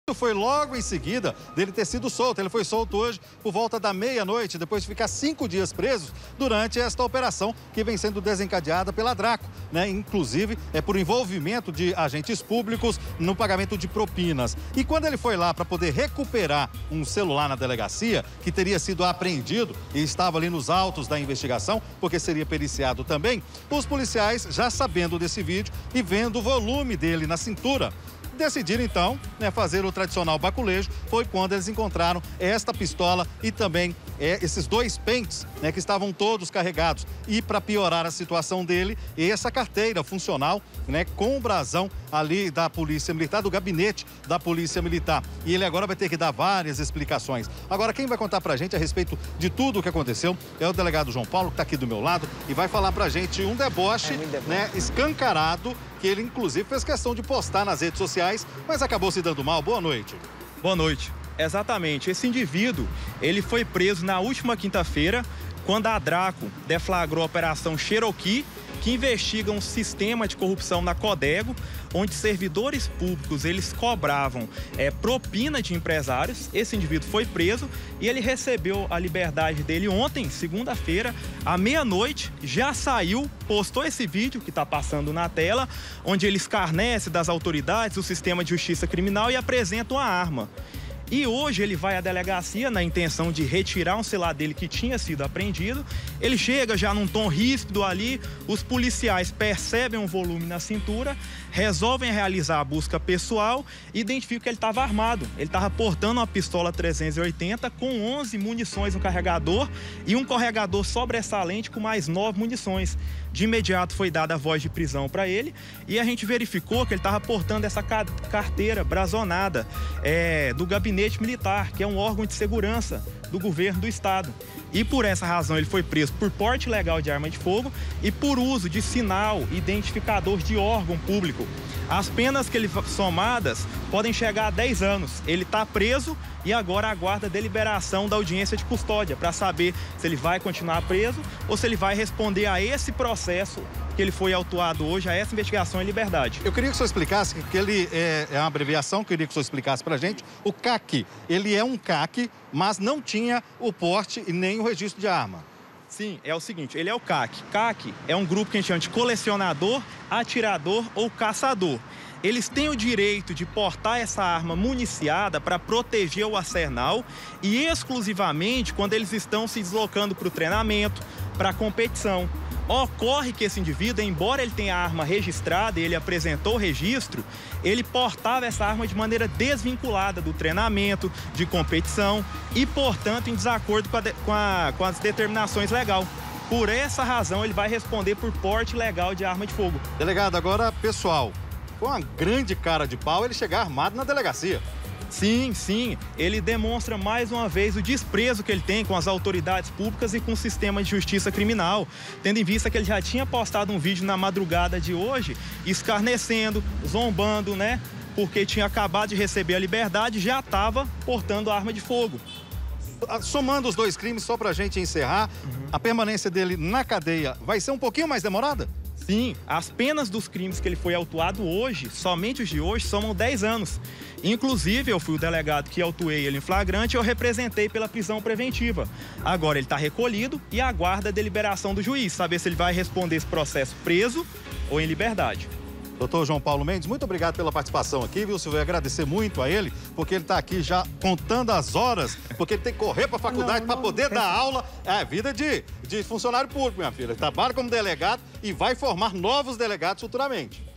DEBOCHADO: HOMEM PRESO DUAS VEZES EM MENOS DE UMA SEMANA
UM HOMEM QUE FOI PRESO NA SEMANA PASSADA DURANTE UMA OPERAÇÃO QUE INVESTIGA CORRUPÇÃO NA CODEGO FOI PRESO HOJE DE NOVO. ELE FOI NA DELEGACIA PRA PEGAR OS OBJETOS DELE QUE FORAM APREENDIDOS DURANTE A OPERAÇÃO CHEROKEE. SÓ QUE DESSA VEZ, ELE ESTAVA ARMADO E TIRANDO SARRO DA INVESTIGAÇÃO. NA SEMANA PASSADA ELE FOI PRESO. FOI PRESO DURANTE A OPERAÇÃO CHEROKEE.